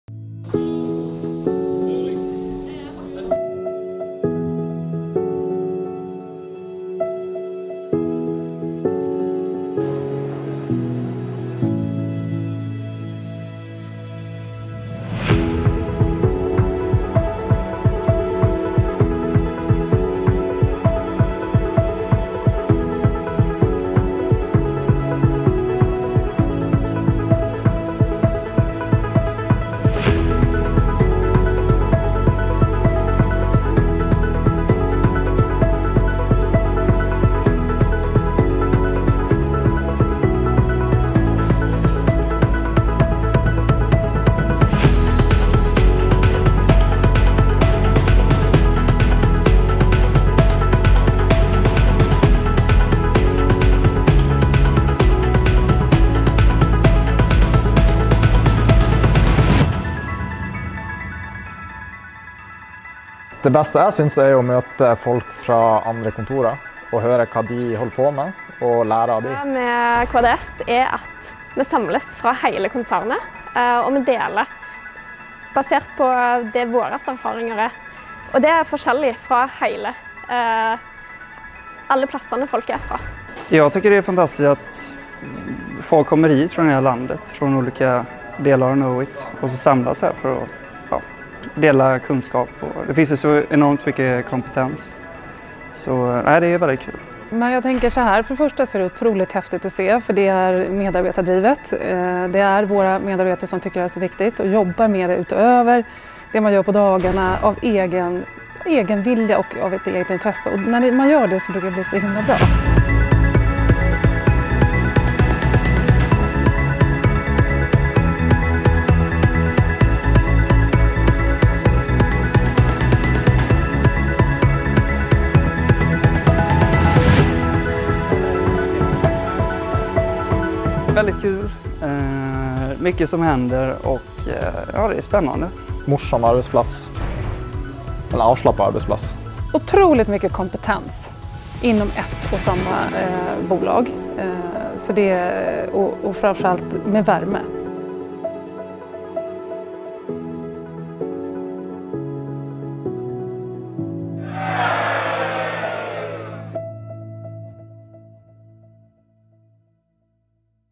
For Developers By Developers - Knowit Developer Summit
For developers by developers! Knowit Developer Summit is the yearly conference that keeps our developers up to date on the latest. Always a wide range of lightning talks, short presentations, long presentations, workshops, and open space discussions. And of course valuable networking across our Nordic regions.